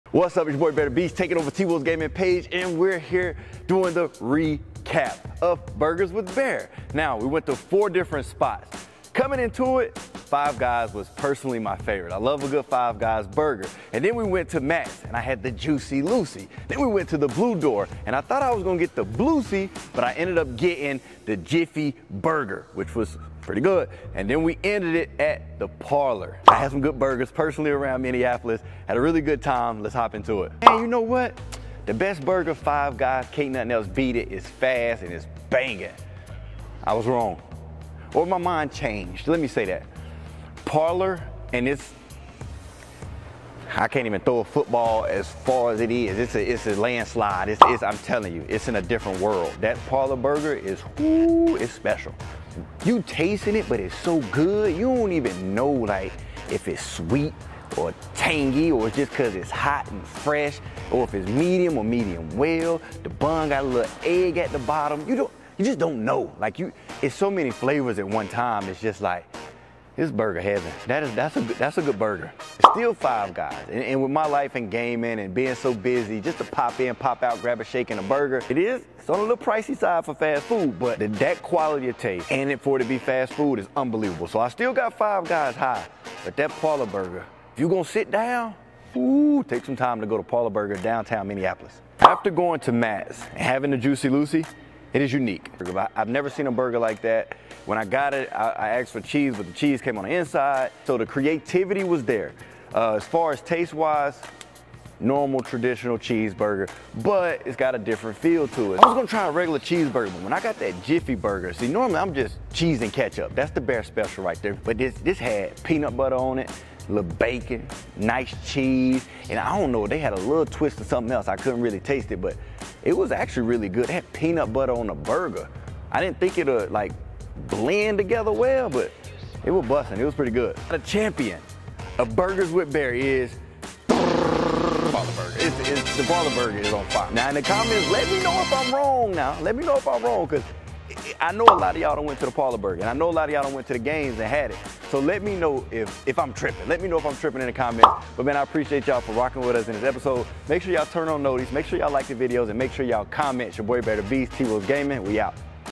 We Reviewed Minnesota's Best Burgers So You Don't Have To
Our very own Burger Expert and NBA 2K Star BearDaBeast has tried a lot of Burgers from Minnesota, and he has some thoughts to share with you!

Bear has some tips for which burgers you should try in Minnesota and what situations you should have these burgers in.

What is your favorite burger in Minnesota? Do you agree or disagree with our burger recommendations? Which burger did we miss?